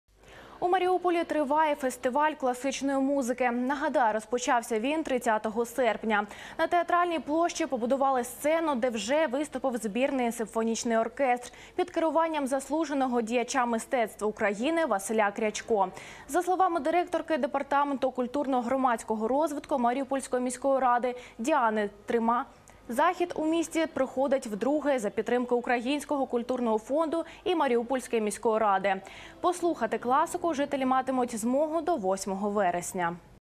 У Маріуполі триває другий фестиваль класичної музики
У Маріуполі триває фестиваль класичної музики. Нагадаємо, розпочався він 30 серпня. На Театральній площі побудували сцену, де вже виступив збірний симфонічний оркестр під керуванням Заслуженого діяча мистецтв України Василя Крячко. Послухати класику жителі матимуть змогу до 8 вересня.